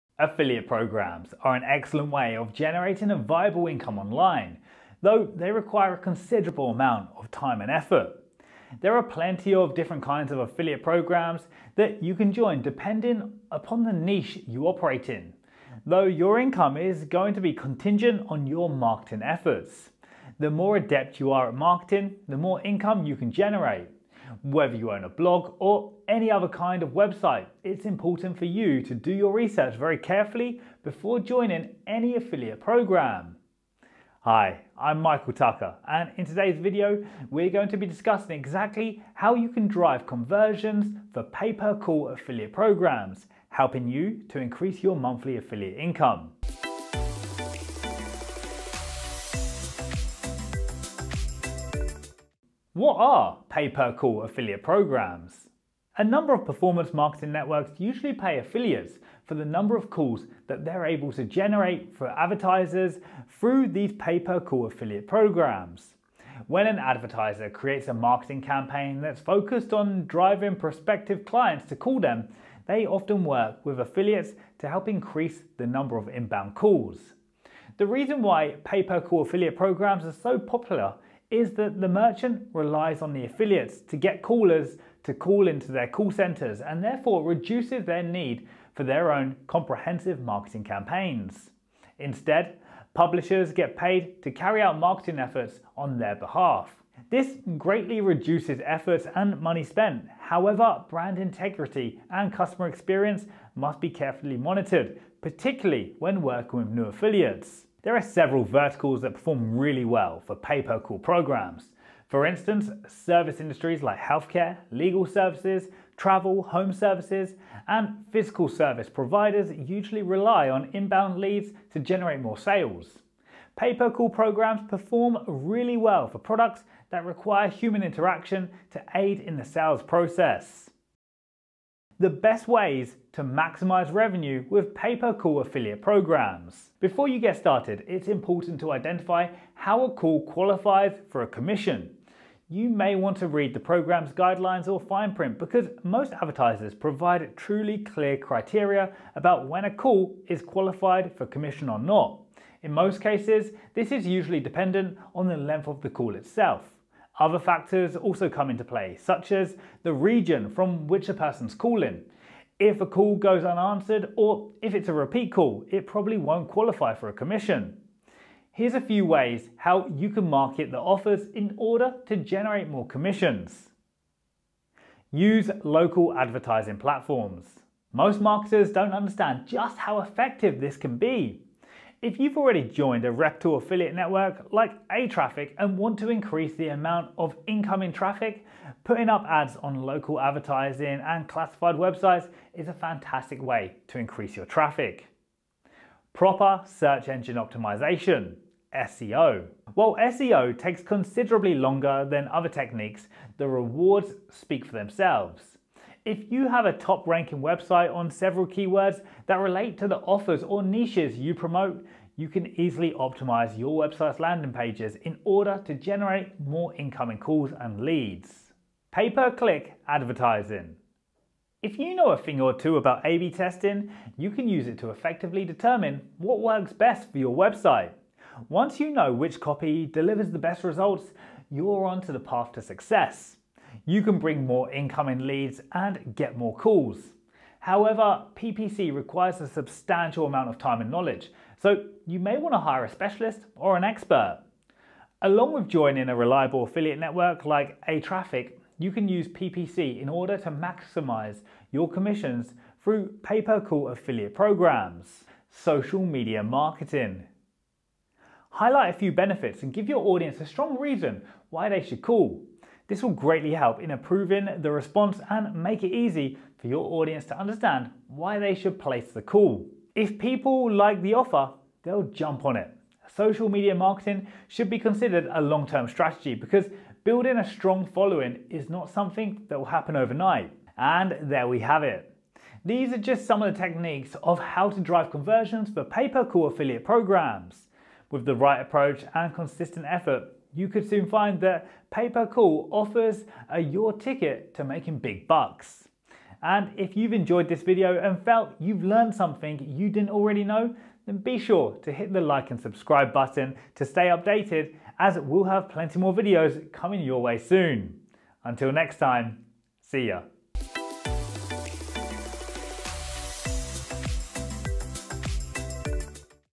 How to Drive Conversions for Pay Per Call Affiliate Programs
Affiliate programs are an excellent way of generating a viable income online, though they require a considerable amount of time and effort. There are plenty of different kinds of affiliate programs that you can join depending upon the niche you operate in, though your income is going to be contingent on your marketing efforts.

The more adept you are at marketing, the more income you can generate. Whether you own a blog or any other kind of website, it’s important for you to do your research very carefully before joining any affiliate program. Most affiliate programs usually pay a commission when a specific task is achieved.